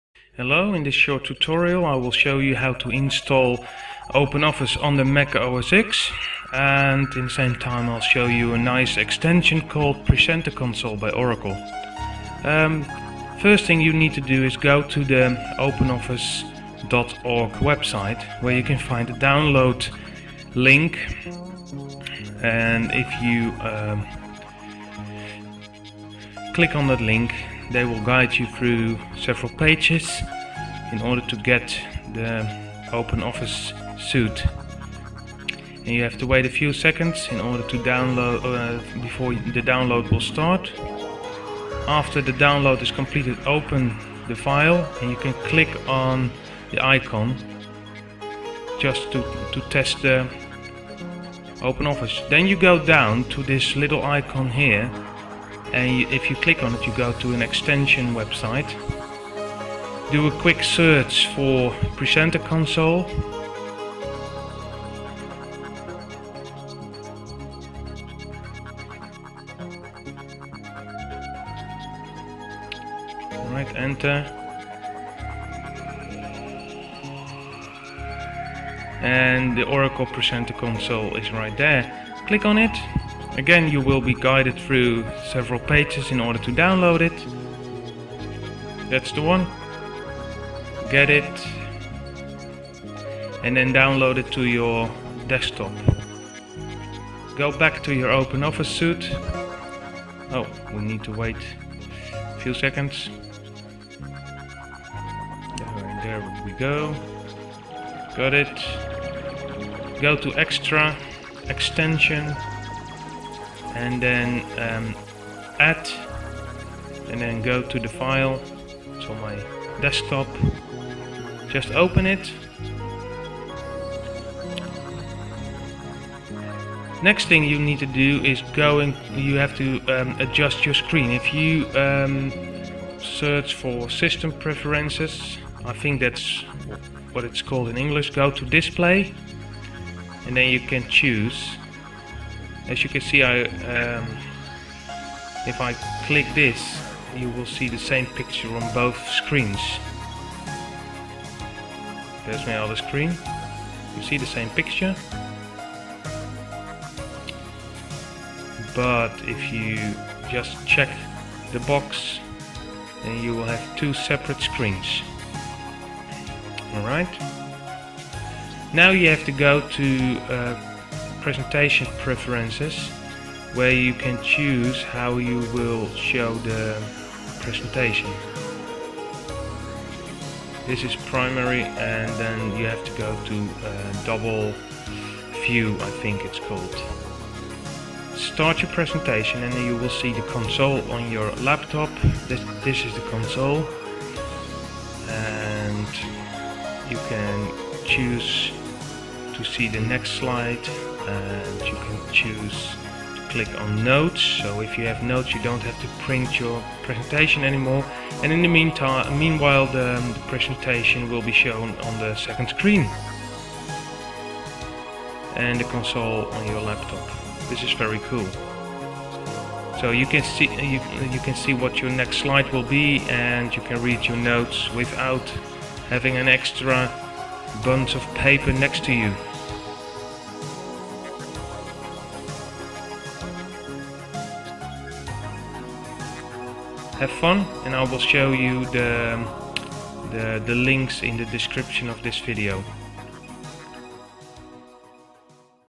Install OpenOffice and Presenter Console from Oracle in Mac OSX
I've made this tutorial for those who want to use OpenOffice as their main office suit.
The Pesenter Console is a very handy extension to use. You don't have to print your presentation anymore because you can read the notes during the presentation.
Look for your self.

I've recorded this with my camcorder, so my apologise for the wobbly view.